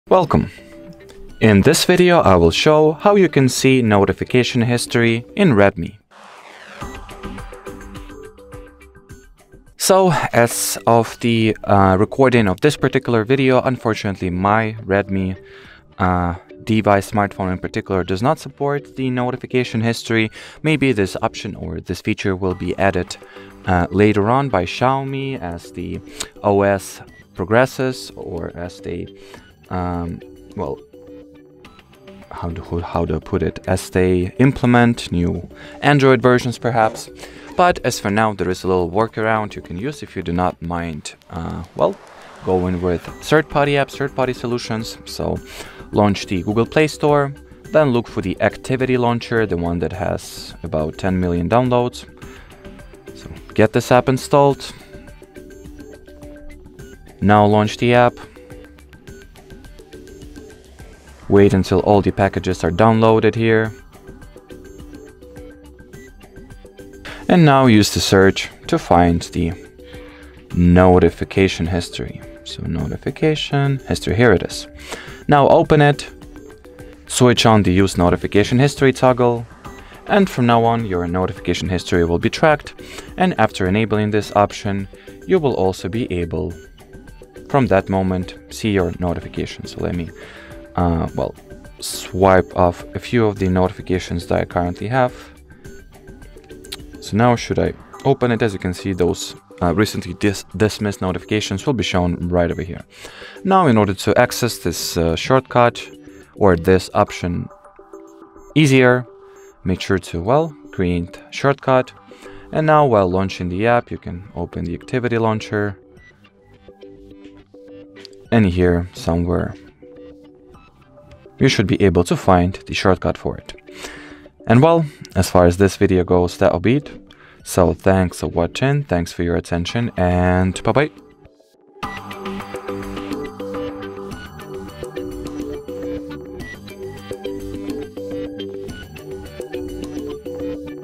How to See Notification History on a Redmi Phone?
Missed an important notification on your Redmi phone? No worries! Redmi devices running MIUI allow you to check your notification history, so you never lose track of alerts, messages, or updates. In this tutorial, we’ll show you exactly how to enable and access notification history on your Redmi smartphone.

How to enable Notification History on a Redmi phone
How to check past notifications even after dismissing them
Tips to manage and customize notifications in MIUI